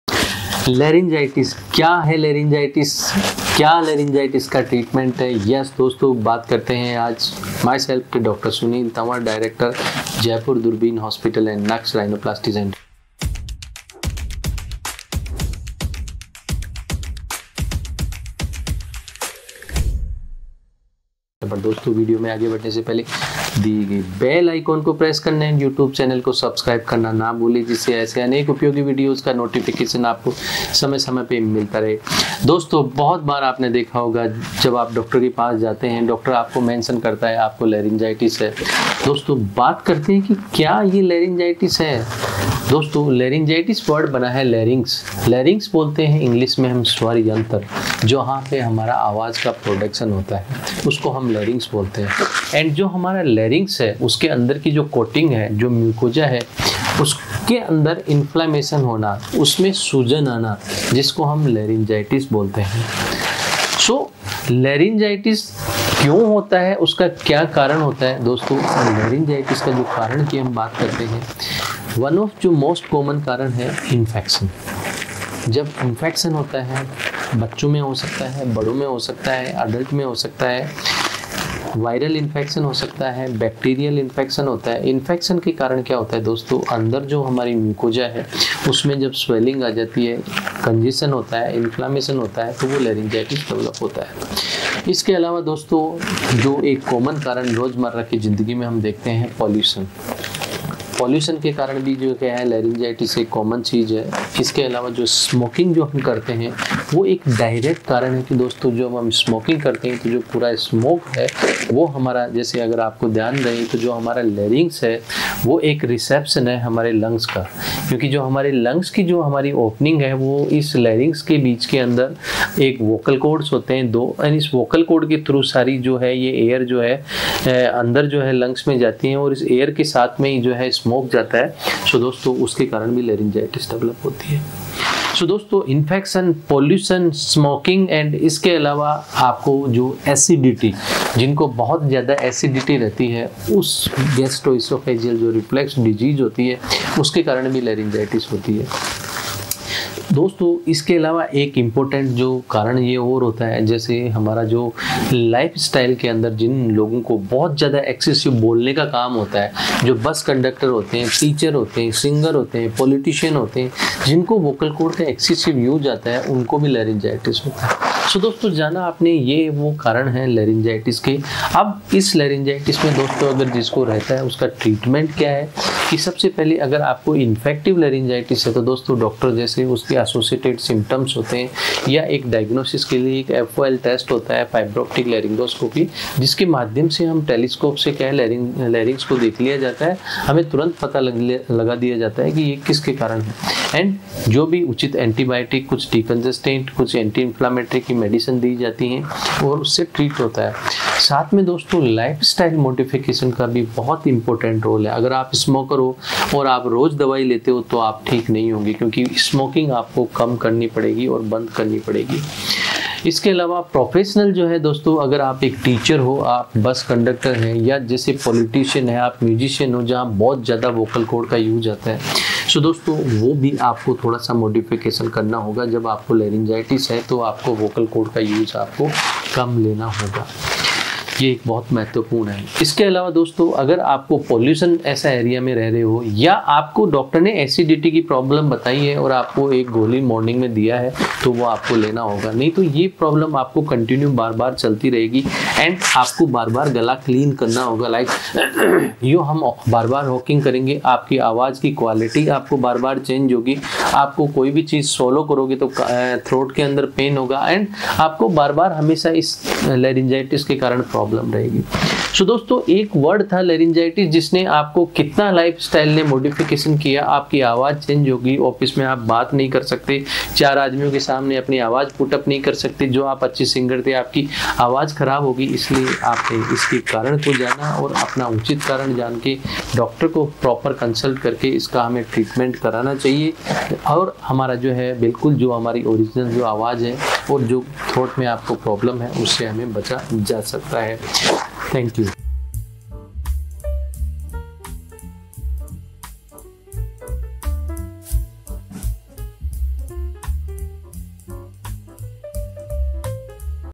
गले की सूजन कारण और स्वास्थ्य | Laryngitis Causes and Recovery | Dr Sunil Tanvar
क्या आपके गले में सूजन या खराश की समस्या है? यह वीडियो सिर्फ आपके लिए है! गले की सूजन Laryngitis के कारणों और उपचारों को बता रहे हैं। अपनी आवाज़ को वापस लाने और गले का दर्द दूर करने का तरीका जानें। अधिक जानकारी के लिए वीडियो देखें।